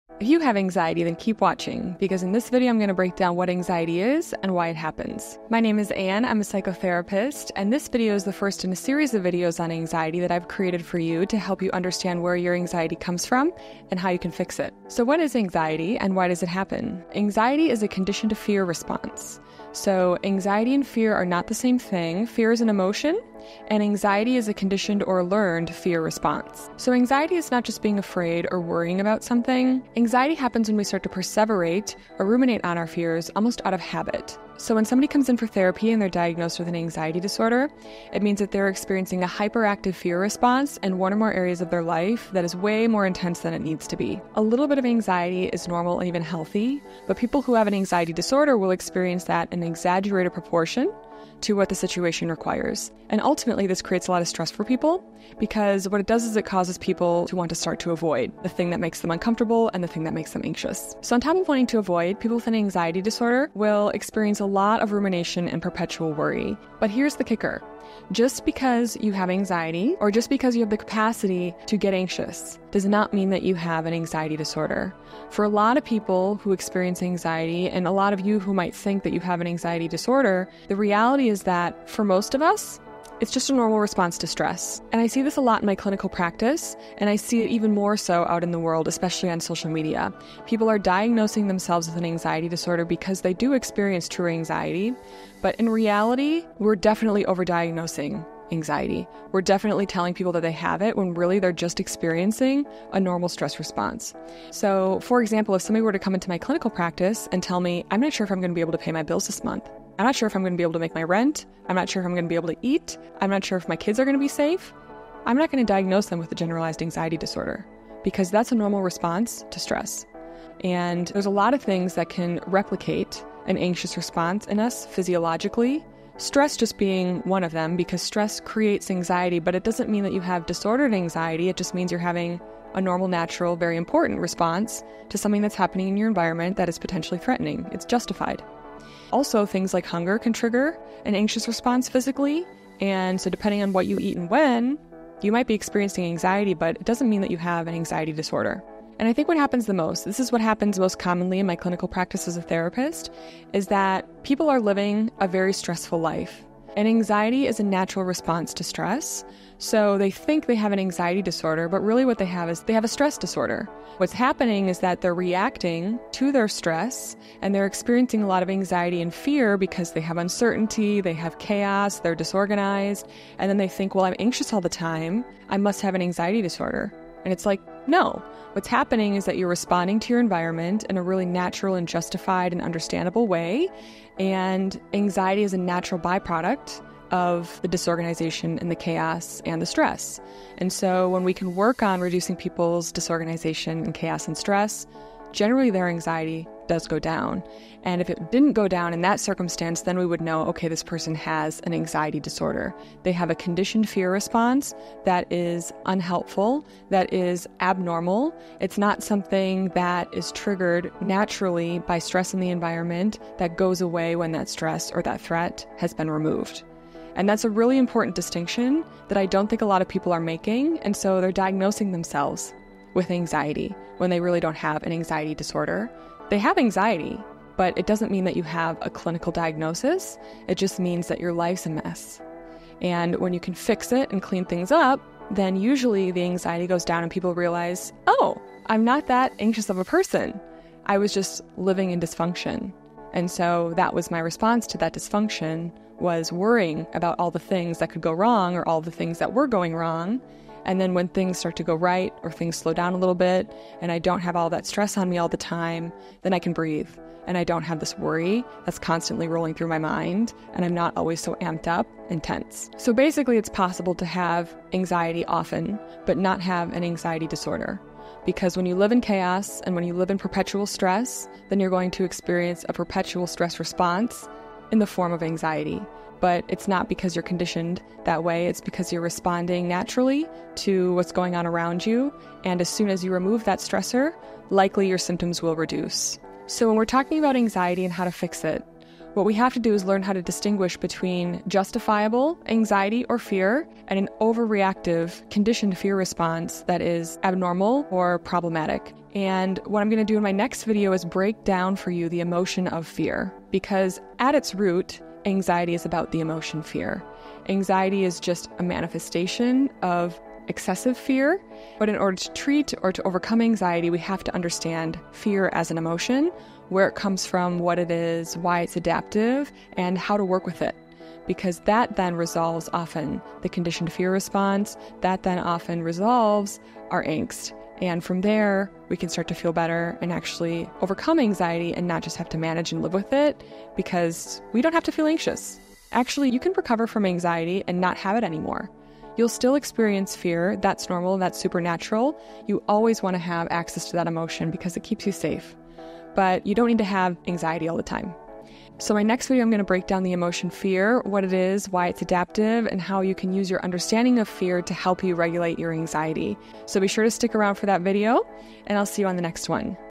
How to Know if You Have Anxiety
Wondering if you have anxiety? This video breaks down how a therapist assesses for anxiety.